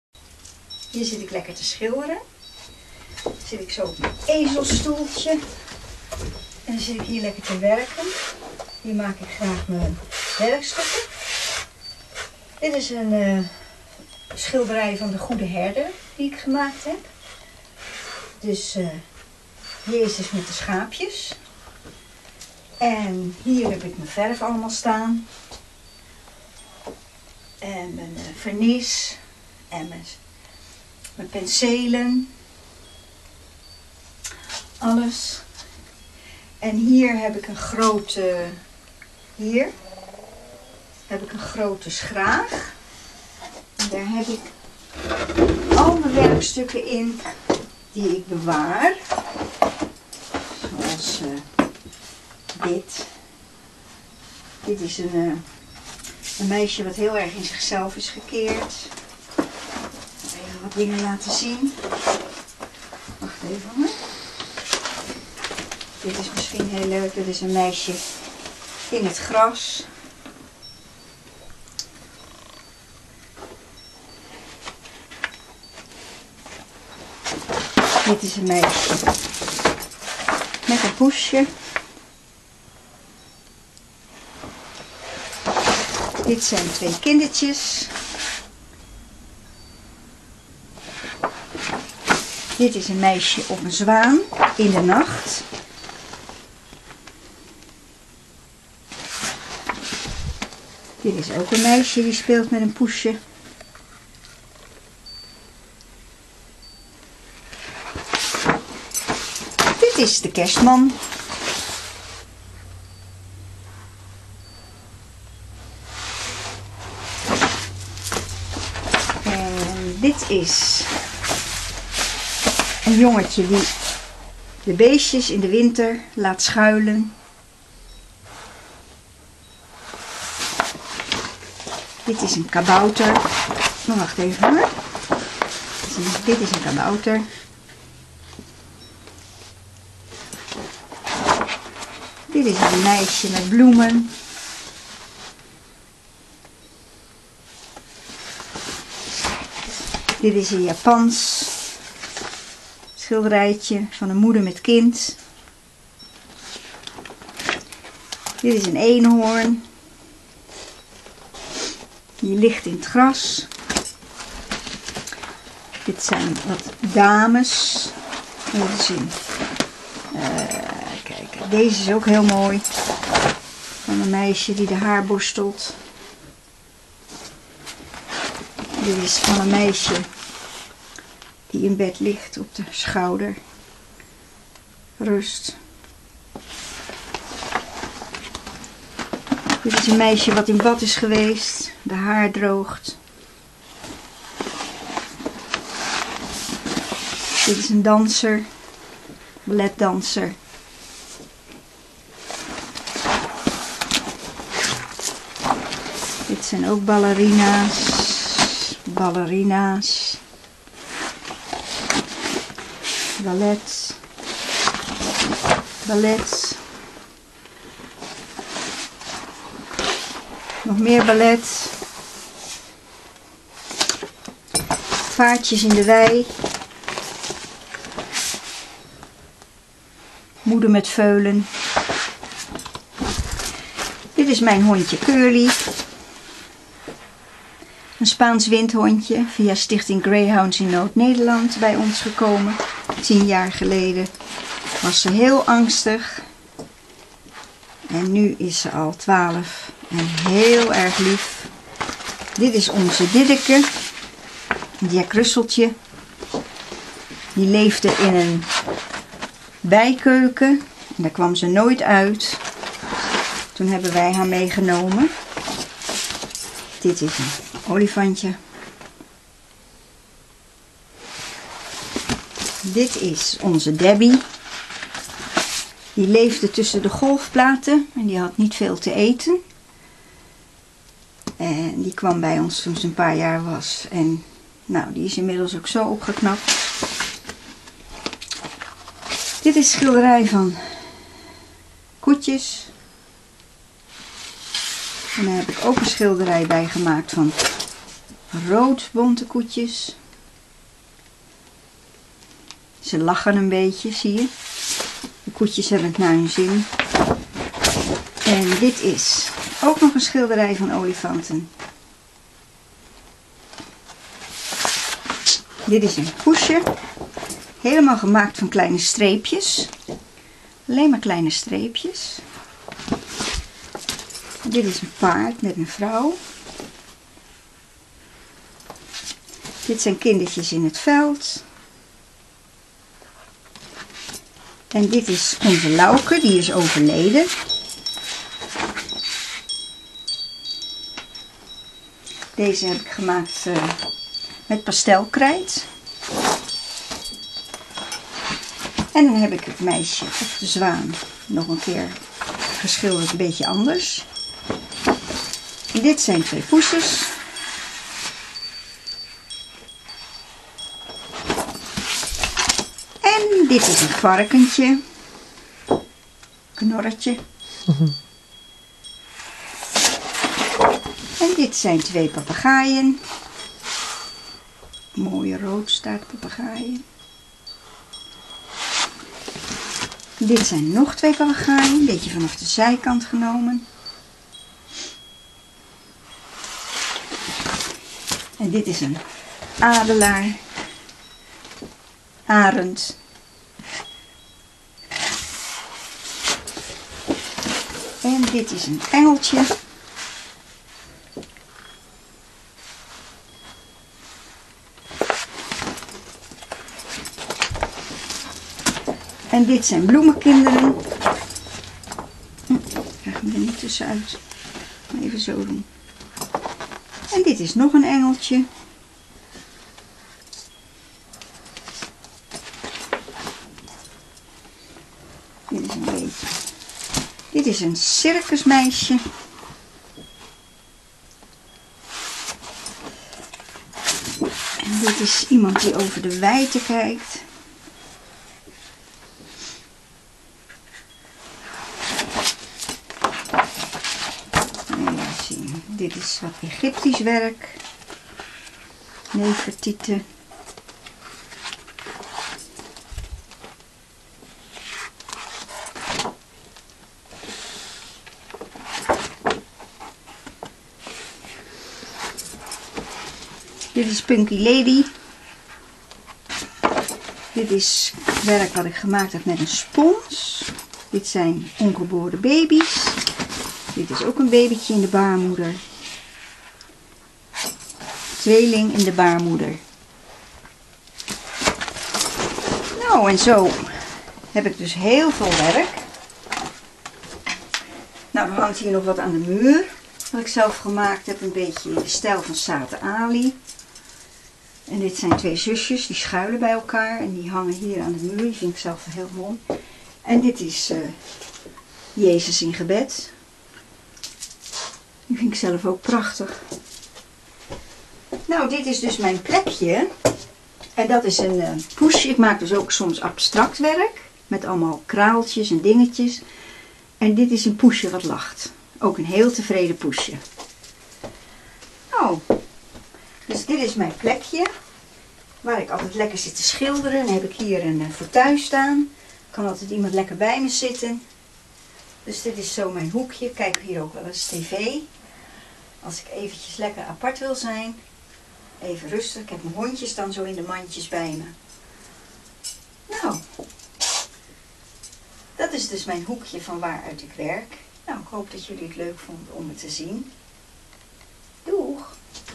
Schilderijen en atelier van Rianne van der Smitte -Groenendijk
filmpje van rianne van der smitte in haar atelier waar zij een aantal werkstukken laat zien.  op haar website  riannegroenendijk.com  of    riannevandersmitte.com   staan nog veel meer werkstukken